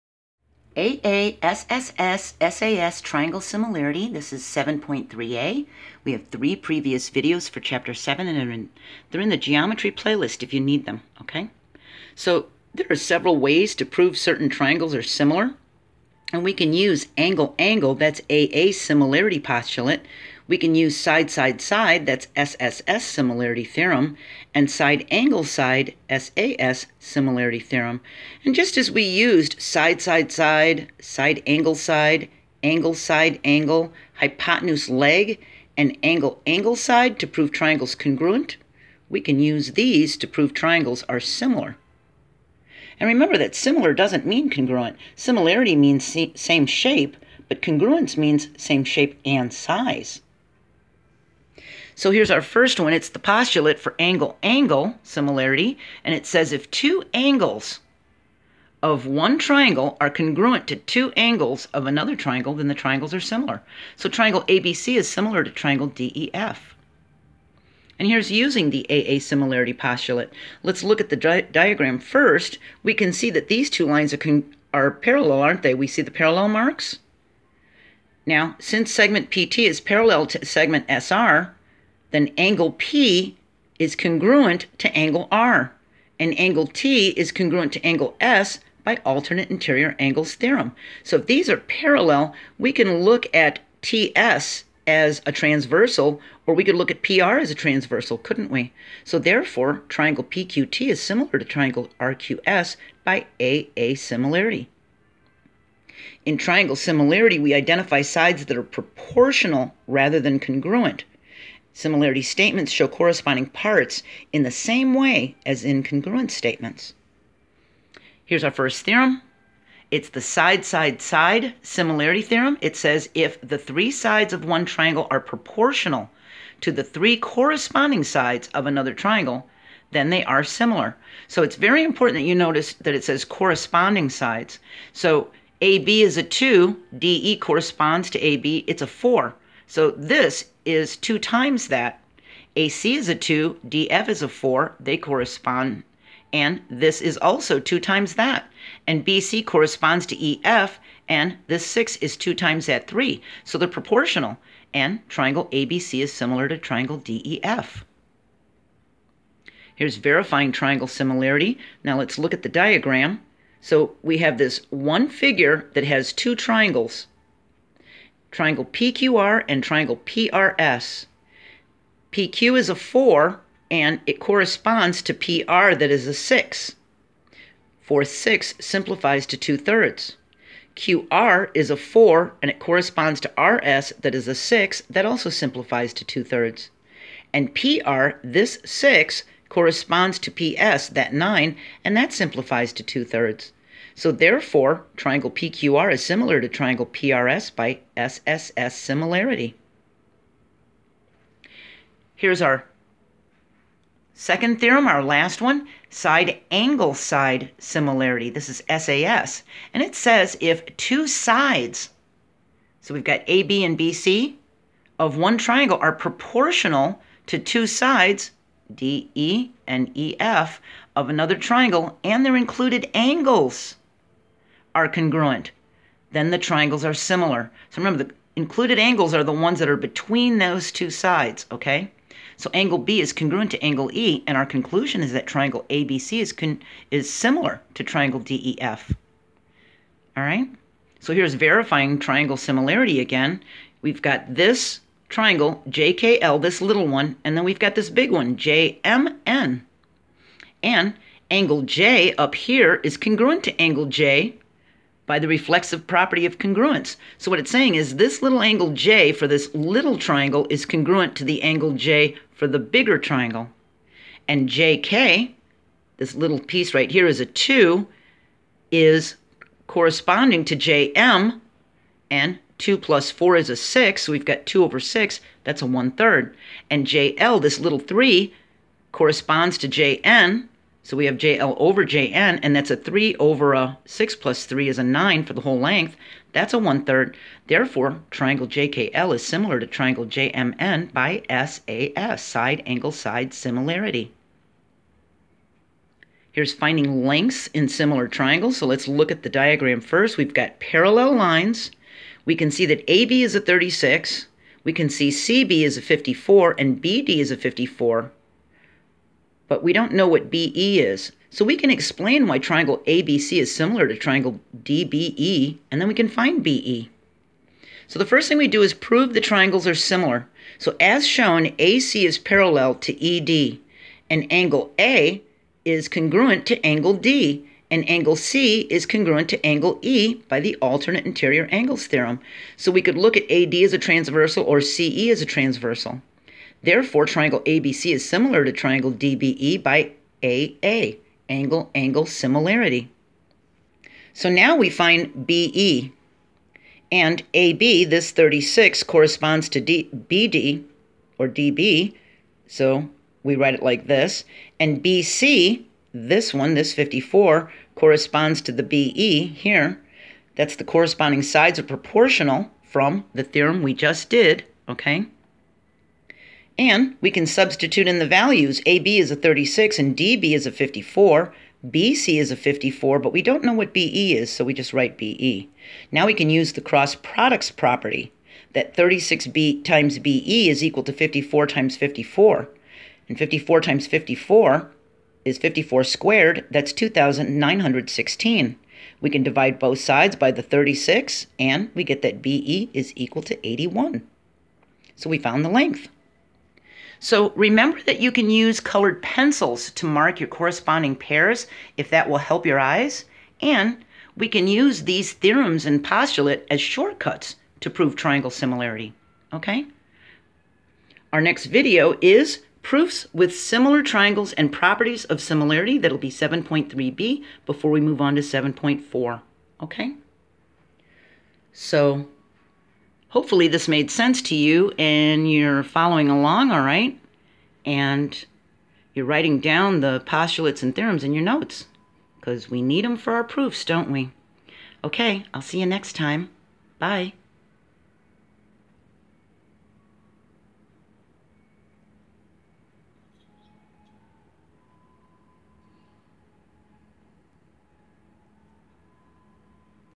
Geometry 7.3a, AA, SSS, SAS Triangle Similarity
An explanation of the Angle-Angle (AA) Similarity postulate, the Side-Side-Side (SSS) Similarity theorem, and the Side-Angle-Side (SAS) Similarity theorem. Explaining why triangles are similar and writing a similarity statement to show corresponding parts. We verify triangle similarity using SSS, and we verify it using SAS. We find lengths in similar triangles by first proving the triangles are similar, then finding the missing length.

MINDS Minds.com/joannsschool

Get a JoAnn's School t-shirt, mug, tote, hoodie,
or inspirational sticker to let everyone know you're a fan!